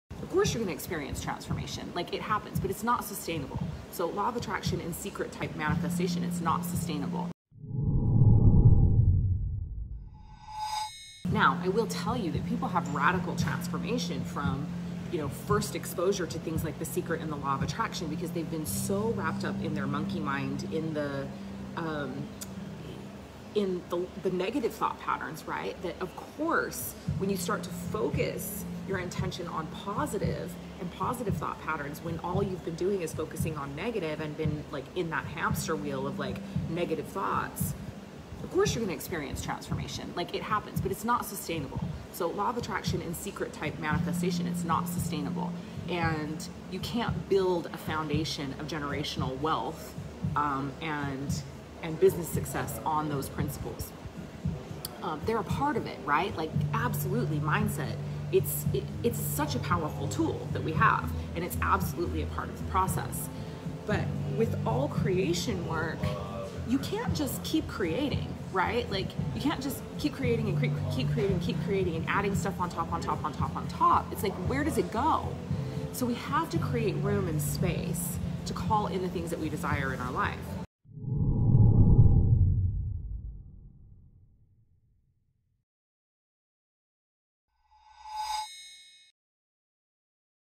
The Law of Attraction is Not Sustainable
You can experience epic results from the Law of Attraction, but they are not sustainable. Do THIS to create that sustainability...watch this video!

To opt-in to Leah's email list and receive a FREE excerpt from her new book Wealth Alchemy